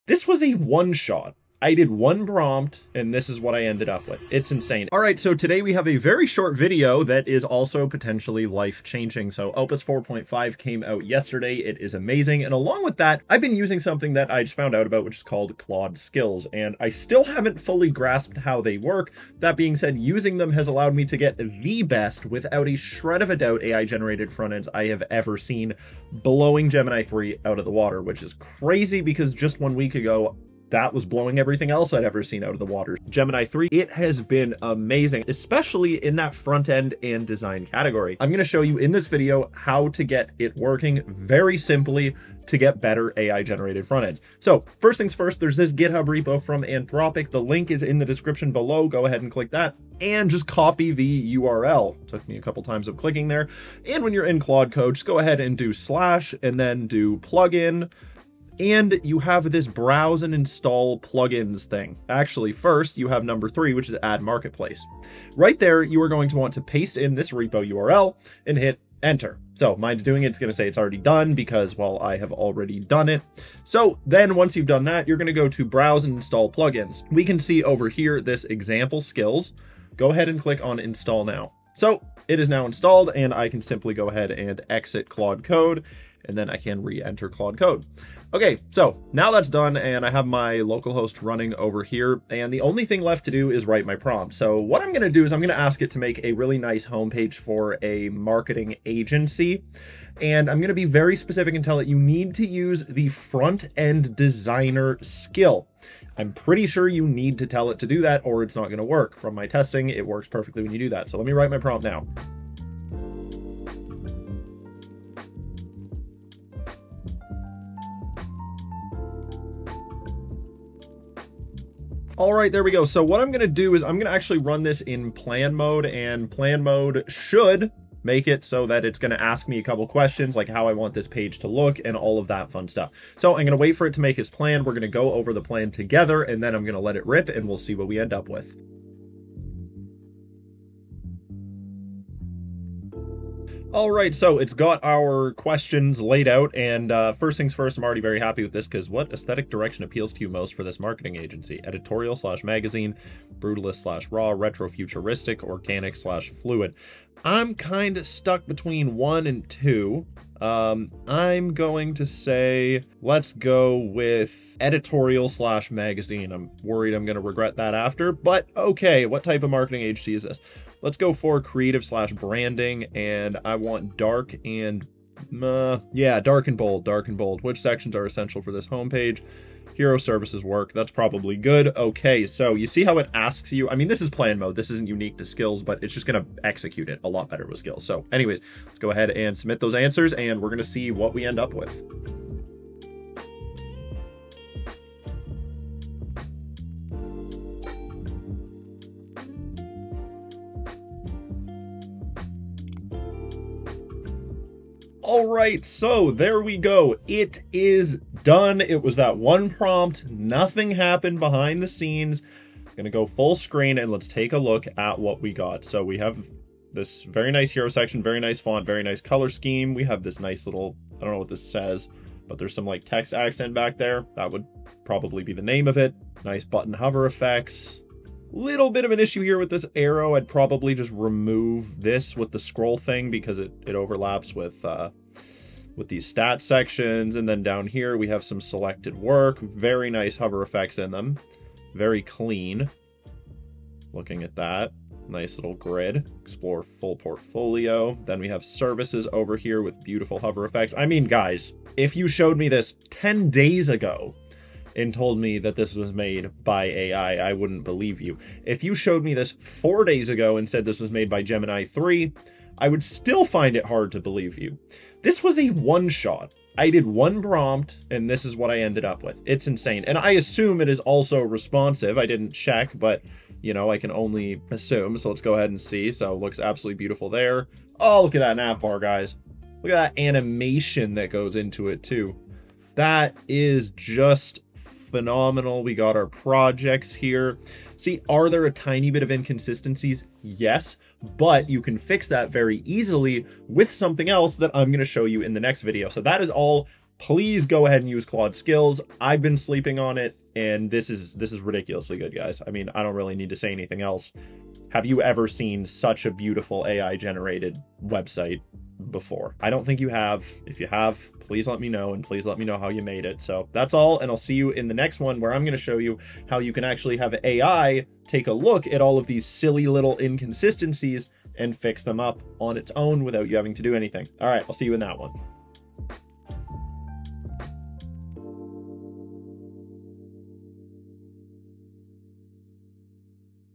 Opus 4.5 With Claude Skills: Perfect AI Front Ends
Unlock the power of Claude 4.5 Opus with its groundbreaking skills feature to generate stunning AI front-ends that outshine Gemini 3. In this quick tutorial, discover how to install Anthropic's plugins and craft professional marketing agency homepages in just one prompt. Revolutionize your web design process with life-changing AI tools today.

TIMESTAMPS:
00:00 Introduction to Claude 4.5 Opus and Skills
00:45 Installing the Plugin from GitHub
01:30 Setting Up Claude Console and Prompting
02:00 Using Plan Mode for Customization
03:00 Reviewing the Generated Homepage
04:00 Demo of Responsive Design and Features
04:30 Conclusion and Next Steps

Claude 4.5 Opus is transforming AI front-end generation, allowing developers and designers to create professional websites faster than ever. By leveraging Anthropic's skills and plugins, you can produce high-quality, responsive homepages for marketing agencies without manual coding, surpassing previous models like Gemini 3 in design sophistication and efficiency.